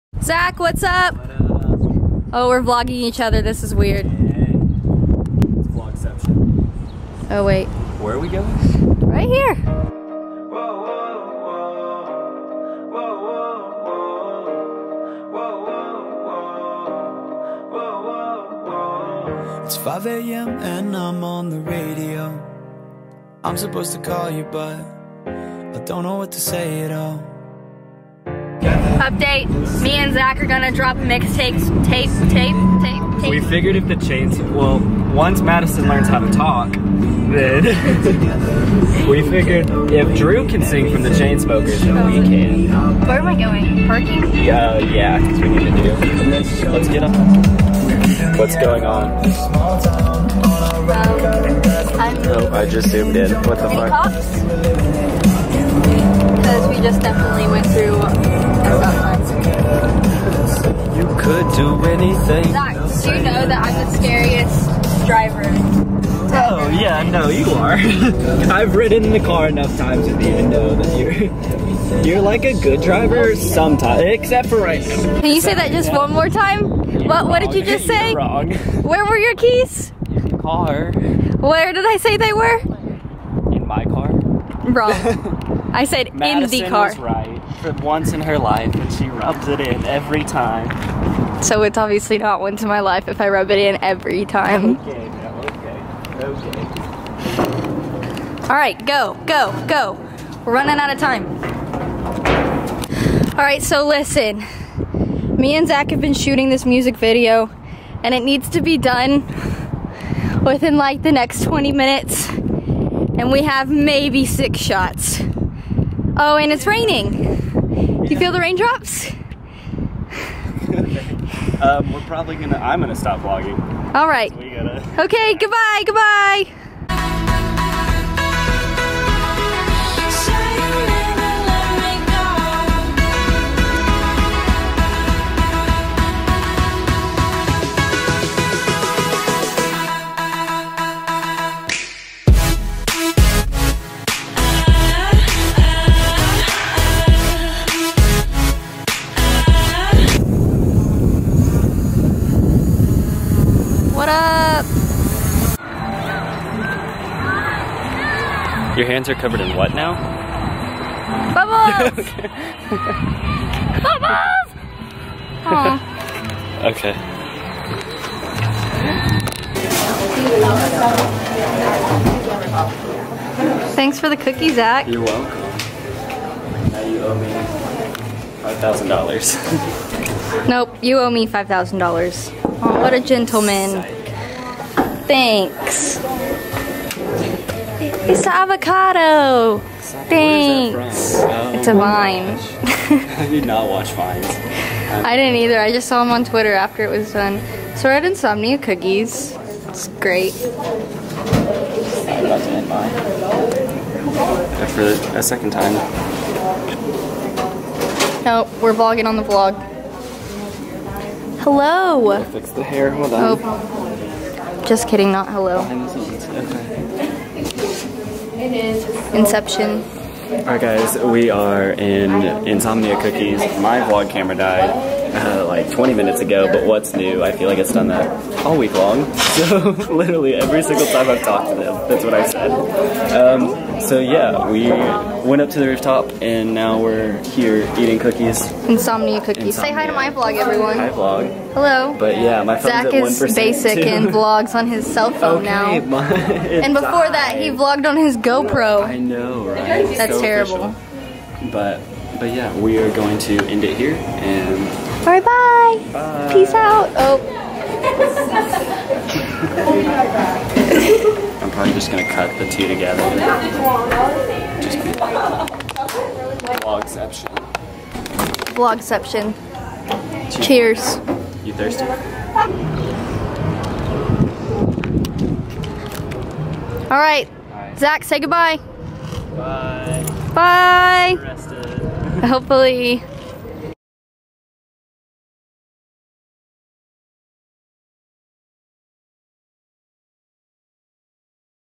THE CHAINSMOKERS TOOK OVER THIS VLOG
A day of fun and shooting led to none other than a chainsmokers takeover